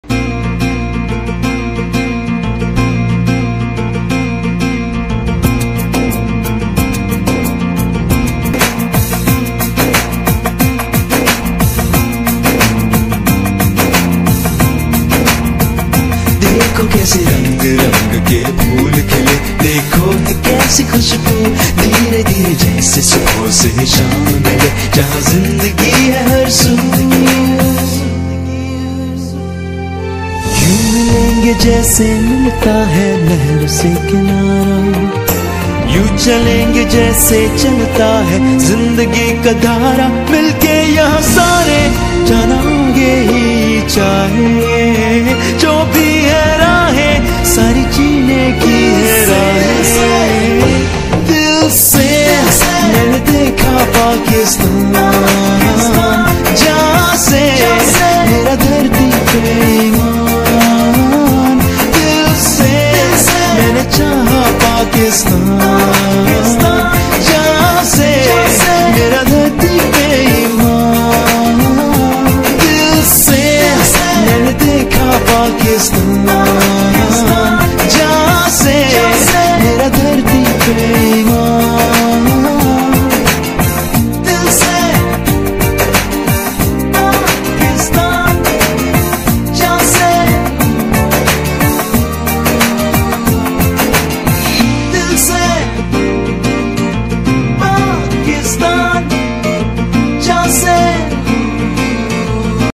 Independence Day flag🇵🇰 Activity fo kids||پینٹ سے آذادی کا جھنڈا بنانا
We have to involve our kids and develop a sense of patriotism in them by doing such kinds of activities.
Hope u will like